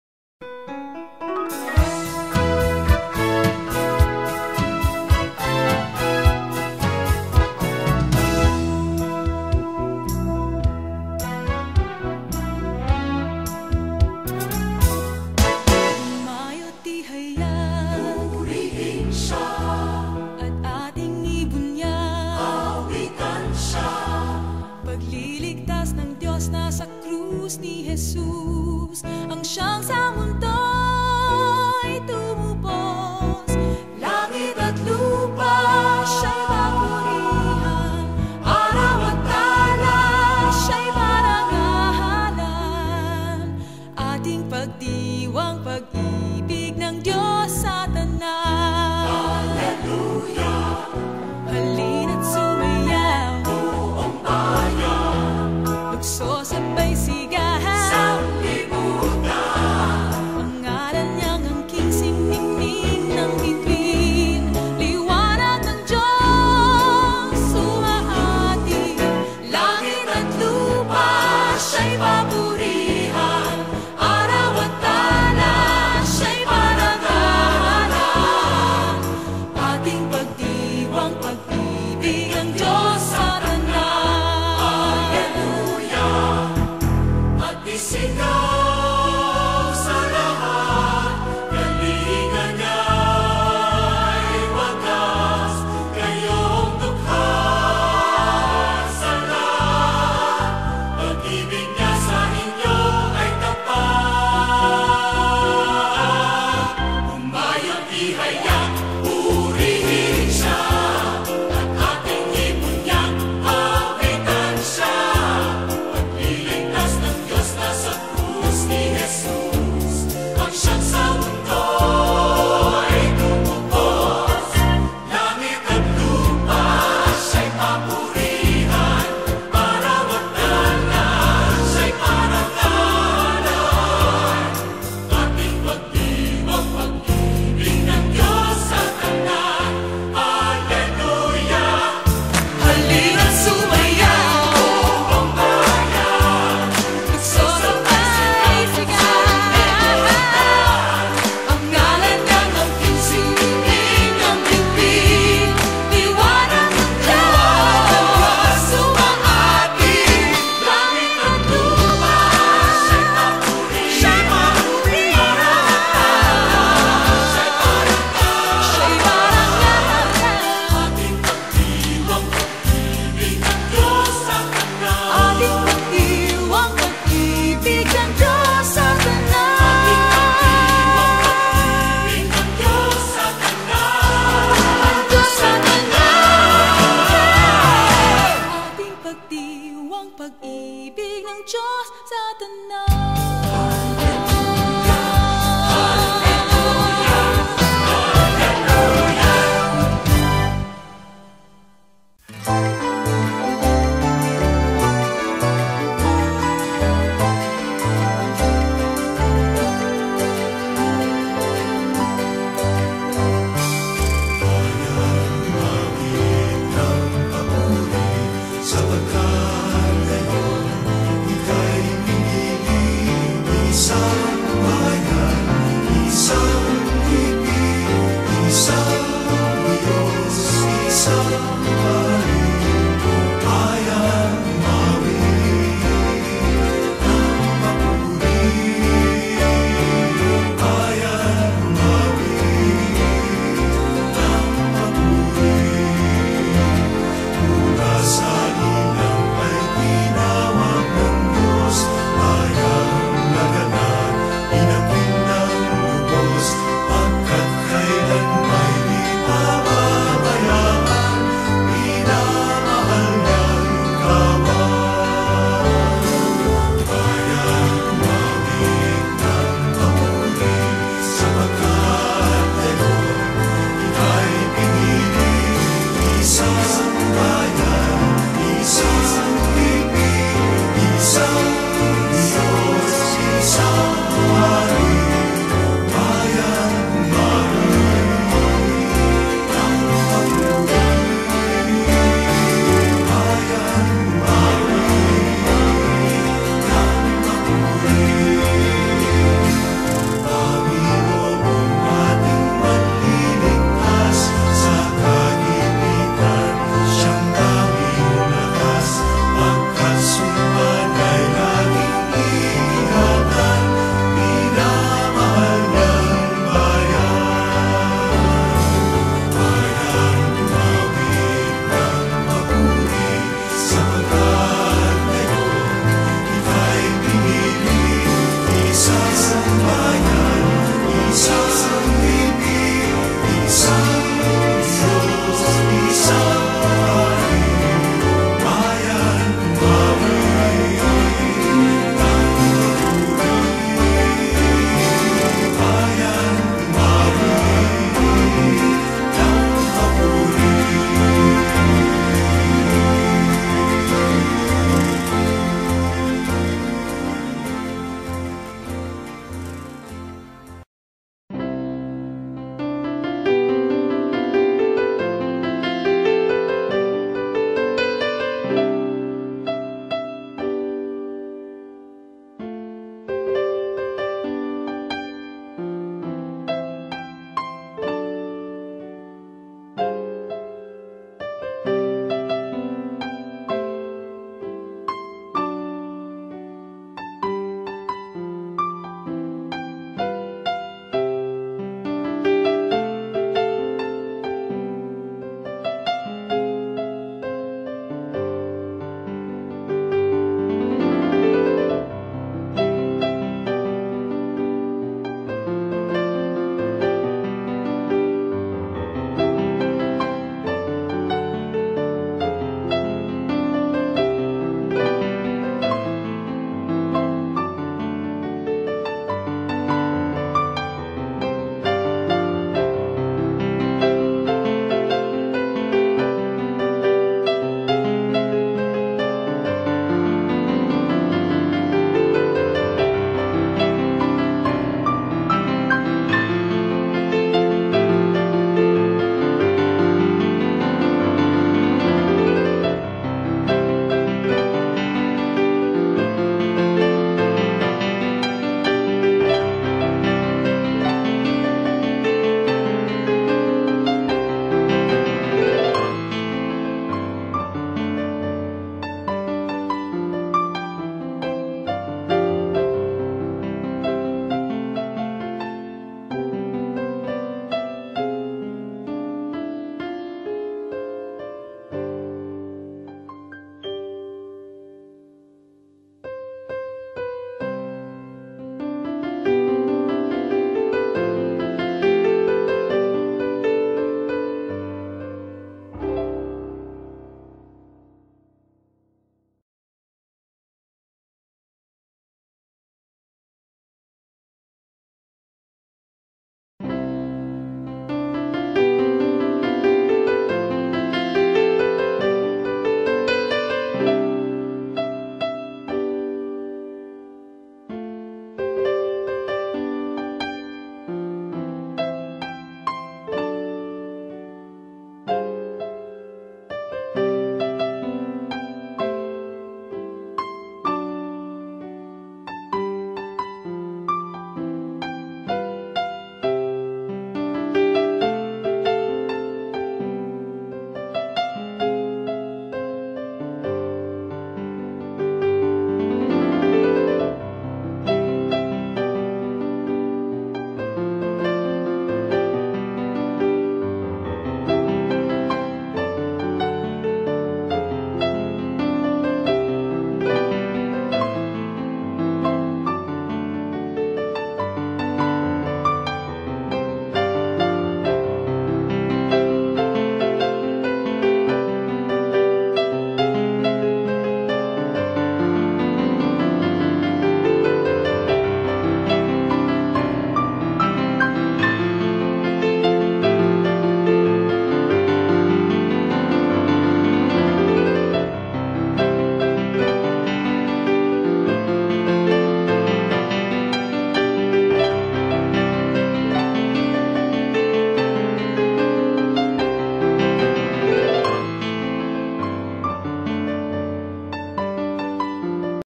PASTORAL VISIT 2009
Pastoral Visit of Most Rev. Florentino G. Lavarias in Immaculate Conception Parish, Barretto, Olongapo City last Nov. 25 & 26, 2009...